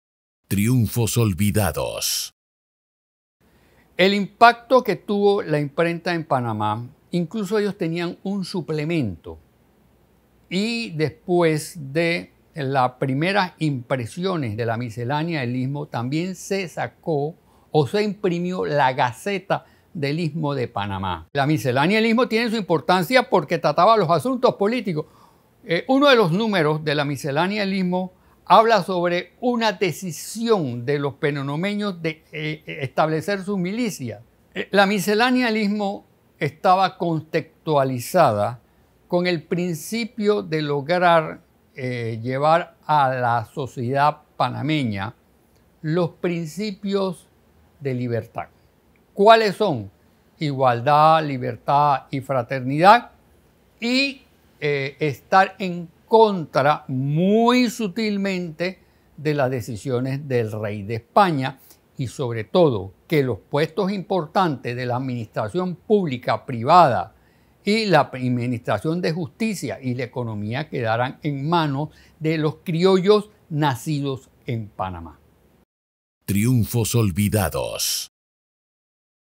TRIUNFOS OLVIDADOS: Impacto de la imprenta en Panamá
El impacto que tuvo la imprenta en Panamá incluso ellos tenían un suplemento, y después de las primeras impresiones de la Miscelánea del Istmo también se sacó, o se imprimió la Gaceta del Istmo de Panamá. La Miscelánea del Istmo tiene su importancia porque trataba los asuntos políticos. Uno de los números de la Miscelánea del Istmo habla sobre una decisión de los penonomeños de establecer sus milicias. La Miscelánea del Istmo estaba contextualizada con el principio de lograr llevar a la sociedad panameña los principios de libertad. Cuáles son, igualdad, libertad y fraternidad, y estar en contra muy sutilmente de las decisiones del rey de España, y sobre todo que los puestos importantes de la administración pública, privada y la administración de justicia y la economía quedarán en manos de los criollos nacidos en Panamá.